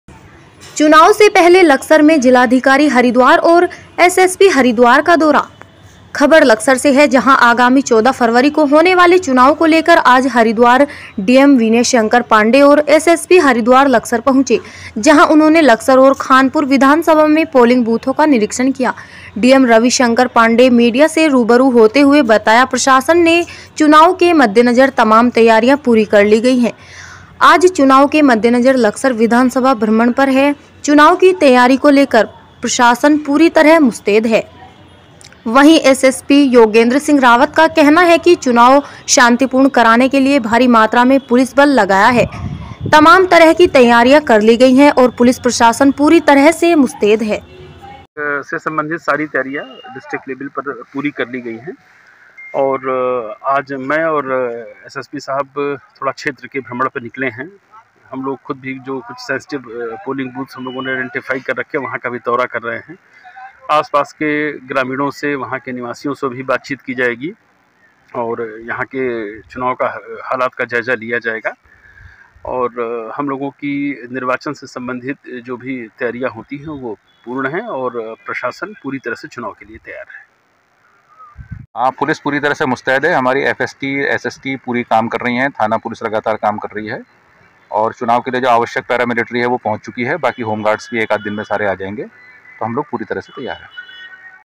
आगामी 14 फरवरी को होने वाले चुनाव को लेकर आज हरिद्वार डीएम विनय शंकर पांडे और एसएसपी लक्सर पहुंचे
चुनाव से पहले लक्सर में  जिलाधिकारी हरिद्वार और एसएसपी हरिद्वार का दौरा

लोकेशन लकसर
संवाददाता गोविन्द चौधरी लकसर।

खबर लक्सर से जहां आगामी 14 फरवरी को होने वाले चुनाव को लेकर आज हरिद्वार डीएम विनय शंकर पांडे और एसएसपी हरिद्वार लक्सर पहुंचे जहां उन्होंने लक्सर ओर खानपुर विधानसभा में पोलिंग बूथों का निरीक्षण किया।
डीएम रवि शंकर पांडे मीडिया से रूबरू होते हुए बताया प्रशासन ने चुनाव के मद्देनजर तमाम  तैयारियां पूरी कर ली गई है आज चुनाव के मद्देनजर लक्सर विधानसभा भ्रमण पर है। चुनाव की तैयारी को लेकर प्रशासन पूरी तरह मुस्तैद है।

बाइट--विनय  शंकर पांडे

वहीं एसएसपी योगेंद्र सिंह रावत का कहना है की चुनाव शांतिपूर्ण कराने के लिए भारी मात्रा में पुलिस बल लगाया है।
तमाम तरह की तैयारियां कर ली गई है और पुलिस प्रशासन पूरी तरह से मुस्तैद है।
बाइट--डॉ योगेंद्र रावत